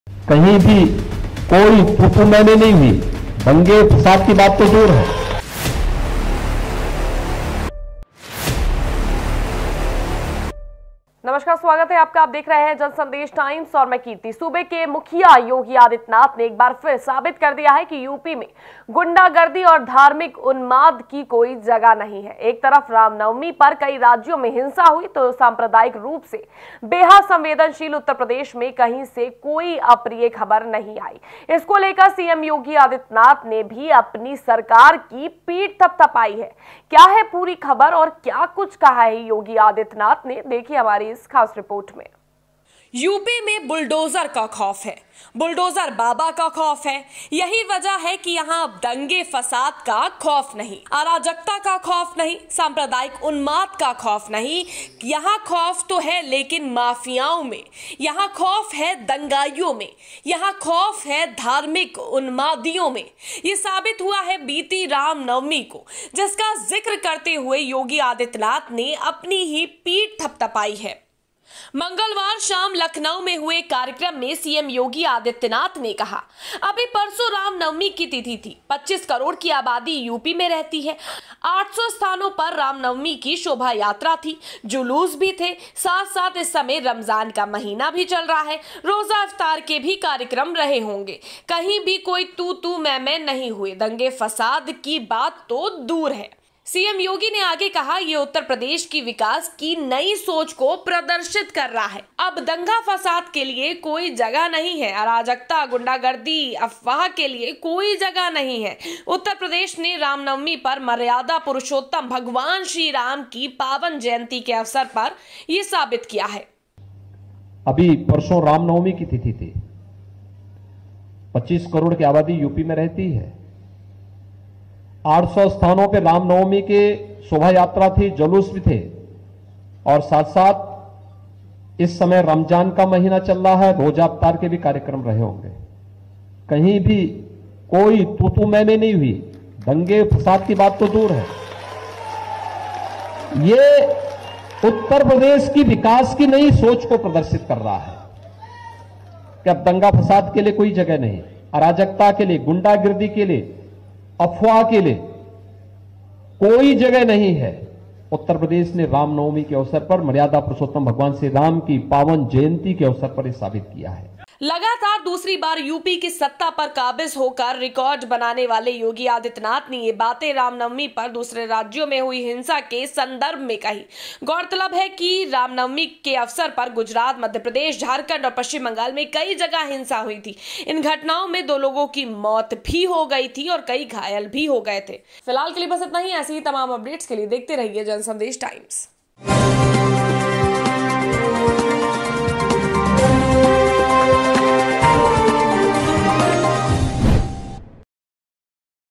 CM YOGI के मुंह से ही सुन लीजिए यूपी में का बा...? ||Uttar Pradesh|Jan Sandesh Times
सूबे के मुखिया योगी आदित्यनाथ ने एक बार फिर साबित कर दिया है कि यूपी में गुंडागर्दी और धार्मिक उन्माद की कोई जगह नहीं है एक तरफ रामनवमी पर कई राज्यों में हिंसा हुई तो सांप्रदायिक रूप से बेहद संवेदनशील उत्तर प्रदेश में कहीं से कोई अप्रिय खबर नहीं आई इसको लेकर सीएम योगी आदित्यनाथ ने भी अपनी सरकार की पीठ थपथपायी है|